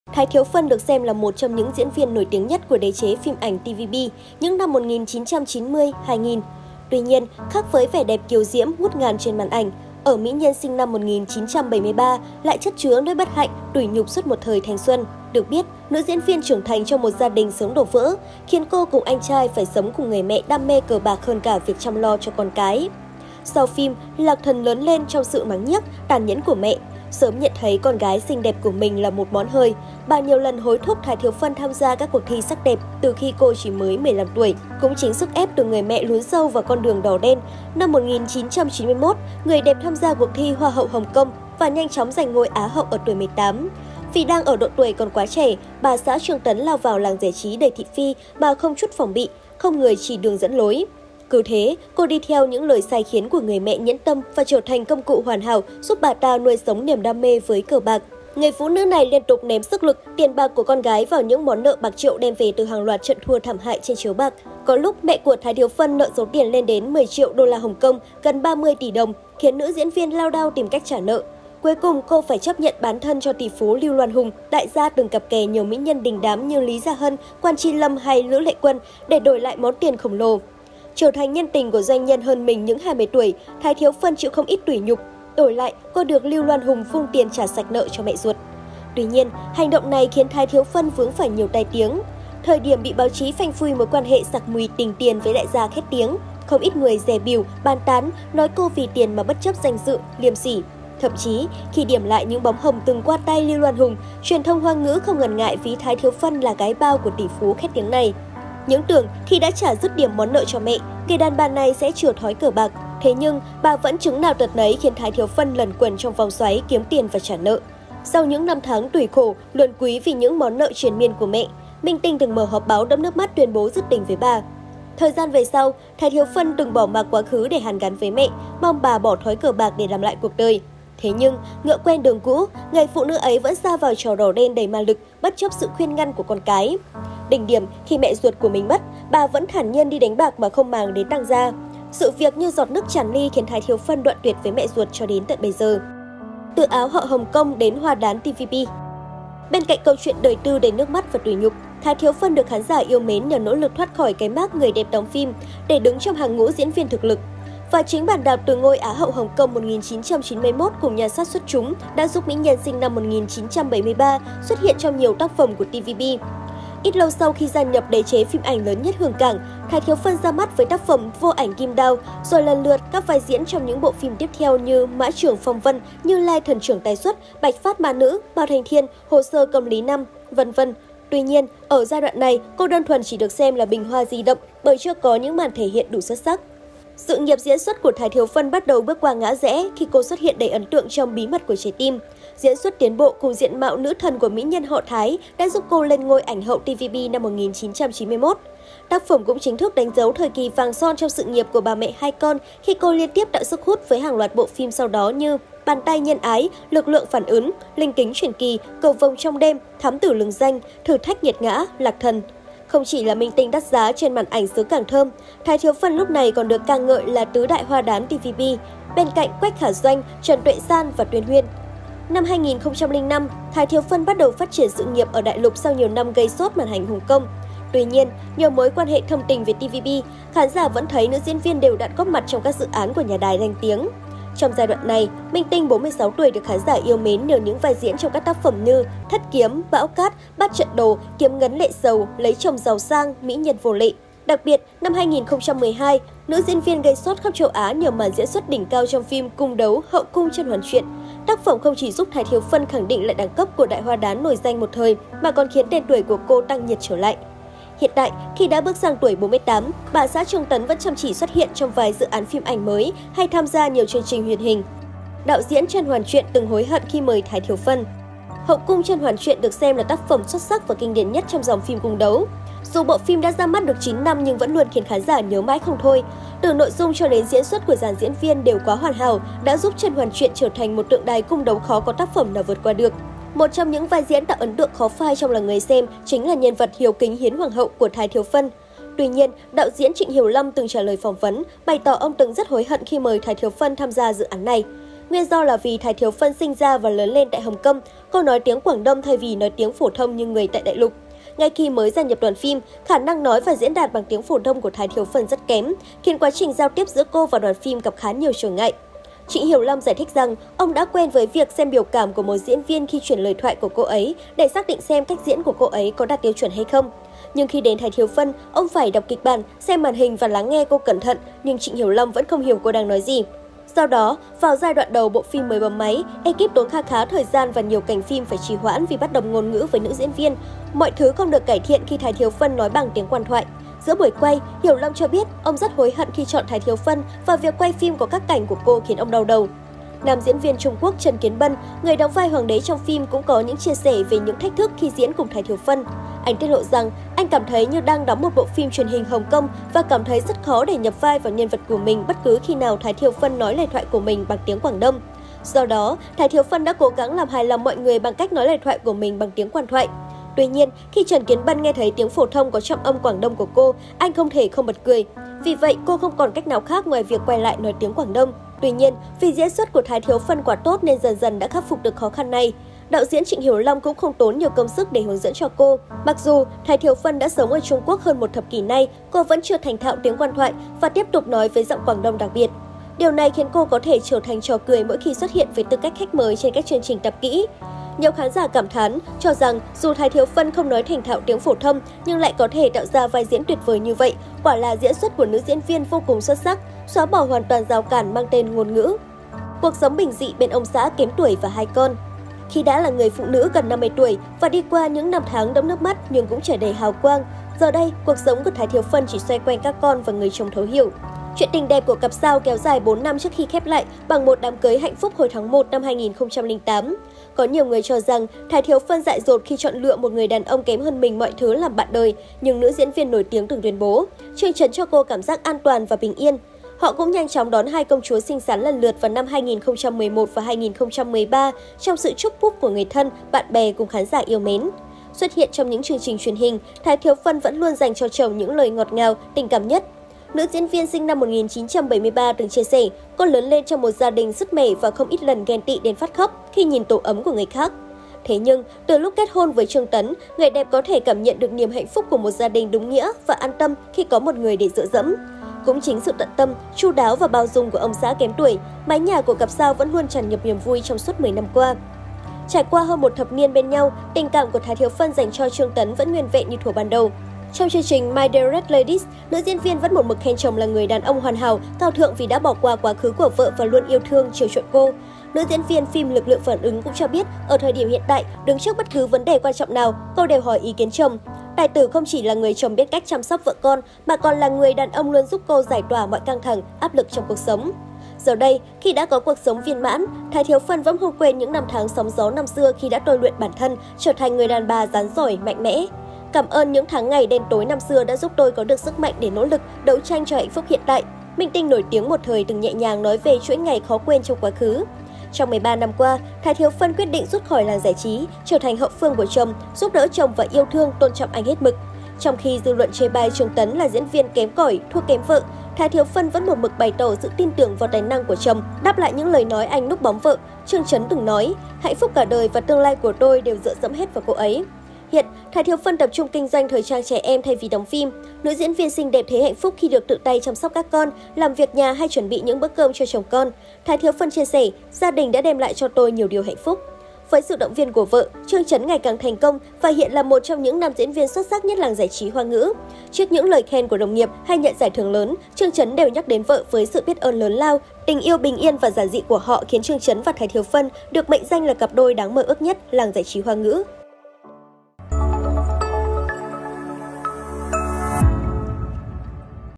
Thái Thiếu Phân - Hoa hậu TVB từng “bán thân” cứu người mẹ đam mê cờ bạc
Thái Thiếu Phân được xem là một trong những diễn viên nổi tiếng nhất của đế chế phim ảnh TVB những năm 1990, 2000. Tuy nhiên, khác với vẻ đẹp kiều diễm, ngút ngàn trên màn ảnh, ở mỹ nhân sinh năm 1973 lại chất chứa nỗi bất hạnh, tủi nhục suốt một thời thanh xuân.